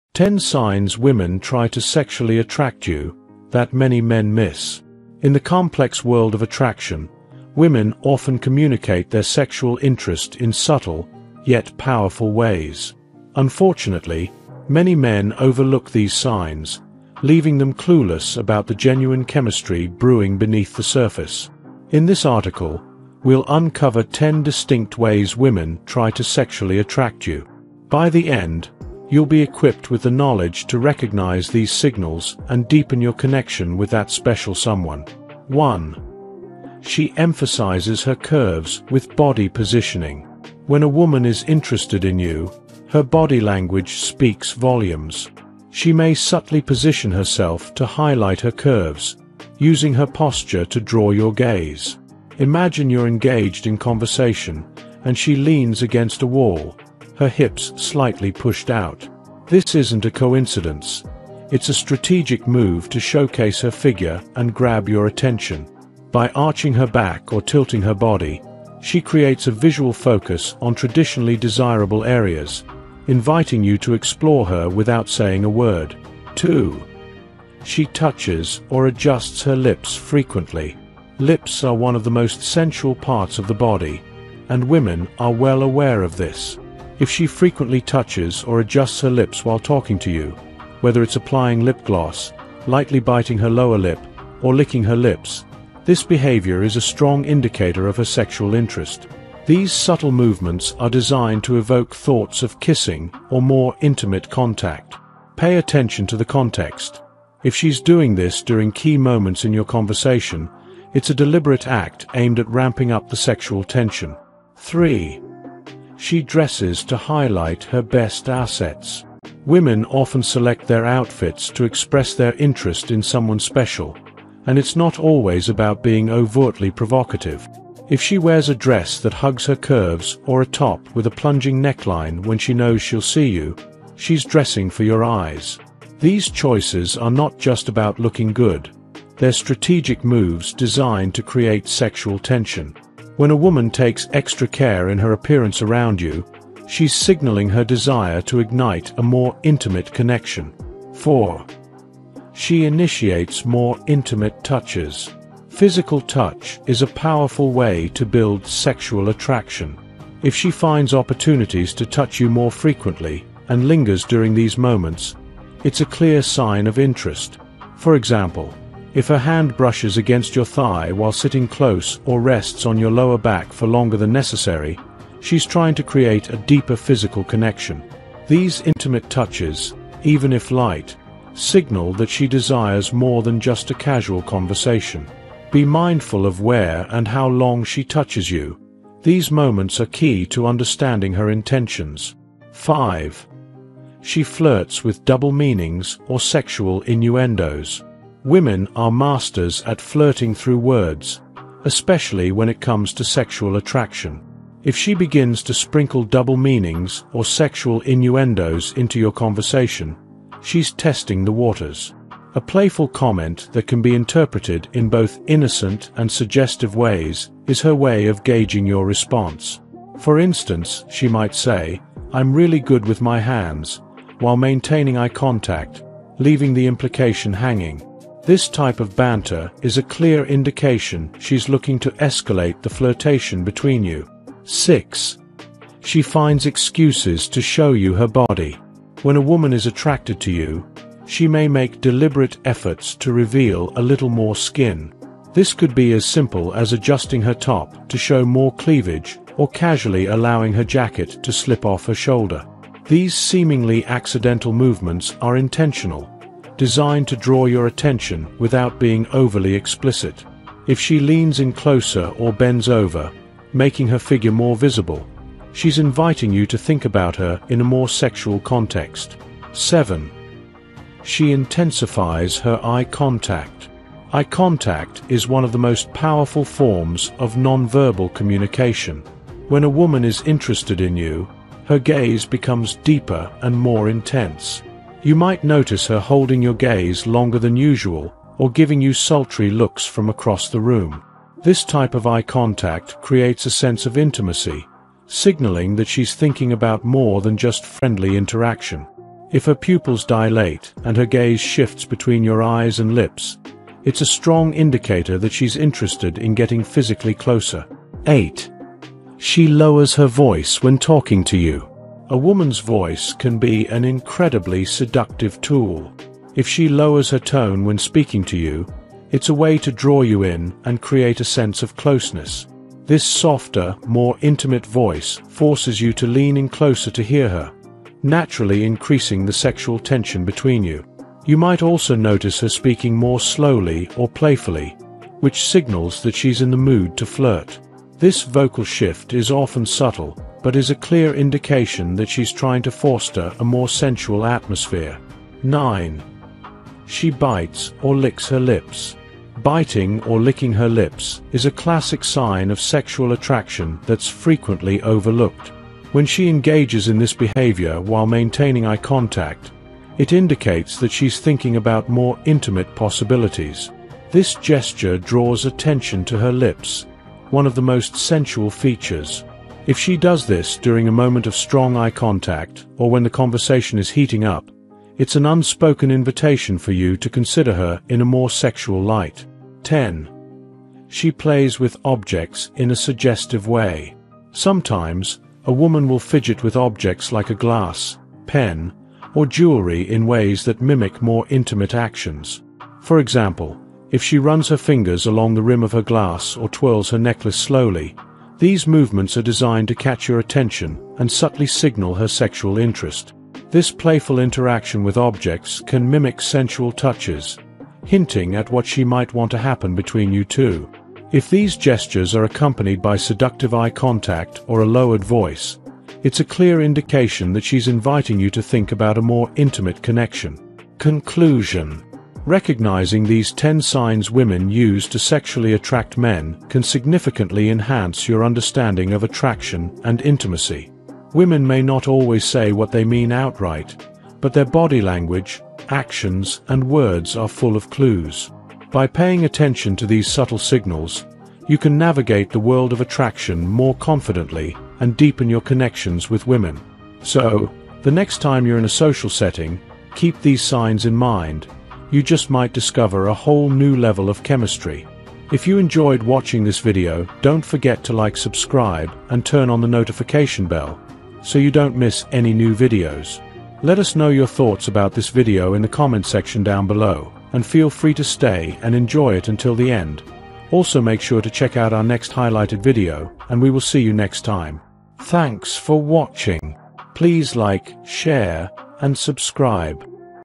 10 Subtle Signs Women Are Sexually Attracted to You – Don’t Miss These Cues | Awesome Facts
10 Subtle Signs Women Are Sexually Attracted to You – Don’t Miss These Cues | Awesome Facts
In the world of dating, understanding attraction can be tricky, especially when it comes to deciphering a woman's signals. Many men often miss the subtle signs that indicate a woman is sexually interested in them. In this video, we’ll break down 10 powerful ways women try to attract you without saying a word. From body language to playful touches, we’ll explore each sign in detail to help you recognize when a woman is igniting sexual chemistry.

Emphasizing Curves: Discover how women use body positioning to accentuate their best features and draw your attention.

Lip Touches: Learn why lip adjustments, like applying lip gloss or biting her lip, are significant signals of attraction.

Dressing to Impress: Find out how clothing choices highlight her assets and communicate her interest in you.

Intimate Touches: Understand the meaning behind those lingering touches and how they create physical connections.

Flirting with Double Meanings: Delve into playful banter and innuendos that test your reactions and build tension.

Revealing Skin: Notice how women find excuses to subtly show more skin to spark your interest.

Lowered Voices: Learn how a softer tone can create intimacy and enhance the sexual atmosphere.

Lip Biting and Licking: Discover the meaning behind these classic gestures of sexual interest.

Playing with Objects: Understand how fidgeting with items can mimic sensual actions and indicate attraction.

By the end of this video, you'll be equipped with the knowledge to recognize these signs and respond accordingly, turning casual encounters into potential romantic connections. Whether you’re navigating the dating scene or looking to enhance your relationship, this guide will help you understand the nuances of female attraction.

So, if you’ve ever wondered whether a woman is interested in you, watch this video to decode the signals she may be sending. Don't miss out on these essential tips to boost your confidence and increase your chances of success in love.

Your support means the world to us and helps us create more content like this.